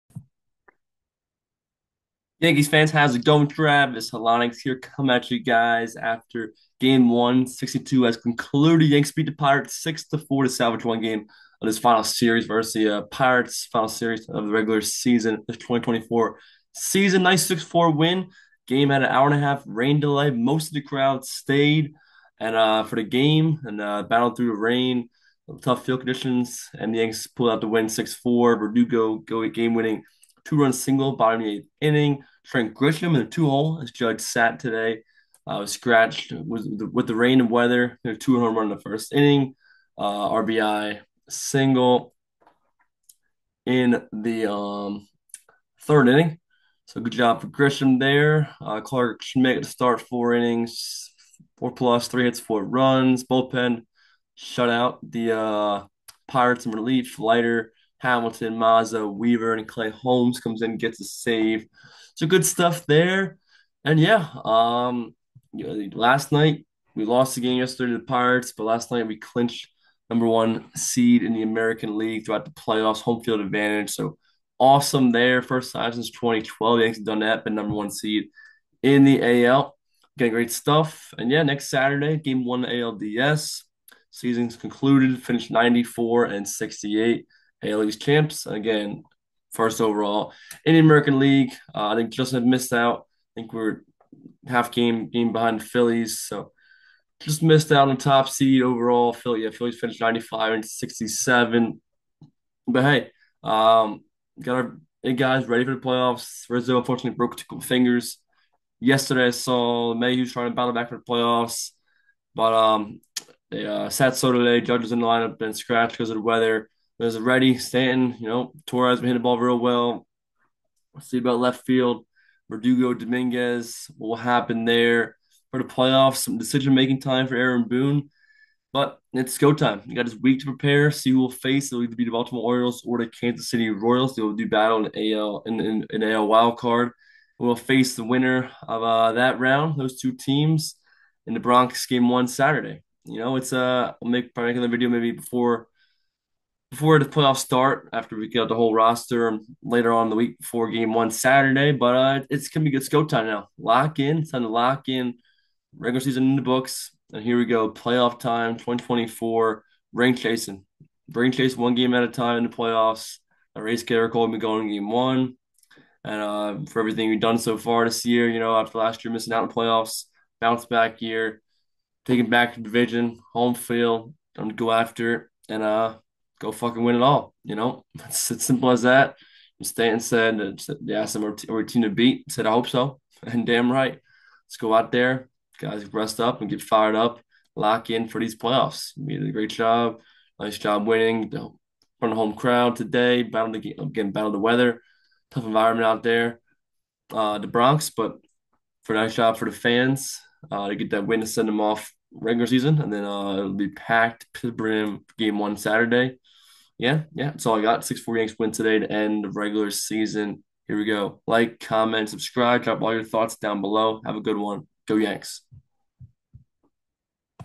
Yankees beat Pirates in game 162 to end the regular season!!
Yankees beat the Pirates 6-4 in game 162 to end the regular season!!! Next up are the playoffs!!! Let's gooo!!! Go YANKEES!!